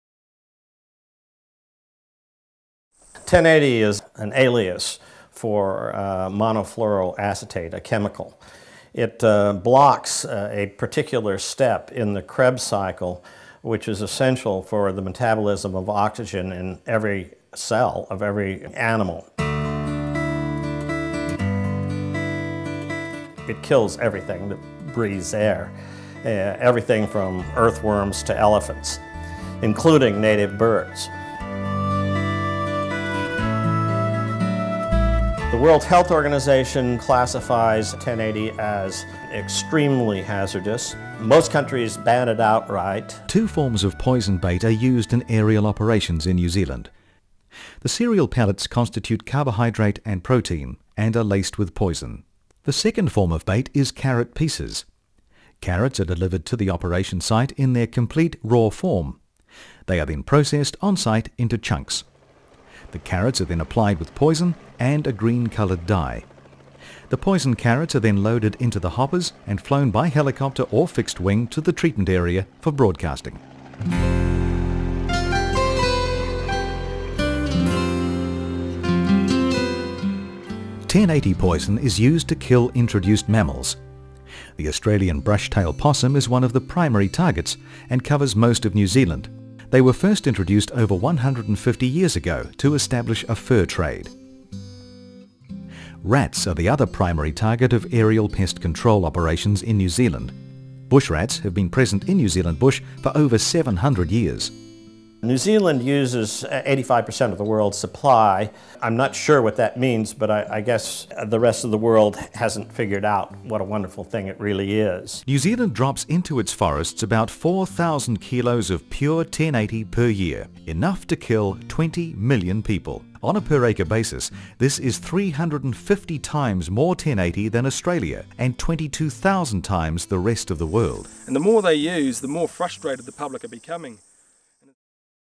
What is 1080 poison?
1080 poison is dropped by helicopters directly into New Zealand forests and streams to kill possums and rats. This short video clip gives an introduction on what 1080 actually is...